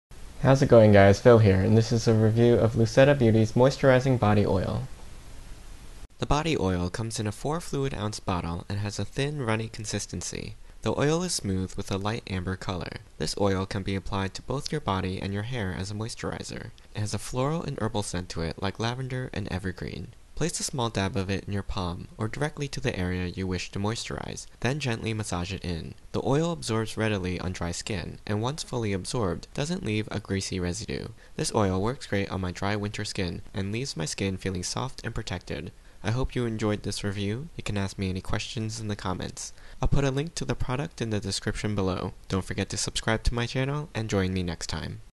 Luseta Olive and Jojoba Body Oil Moisturizer Review
Absorbs quickly, moisturizes and protects, feel light not greasy.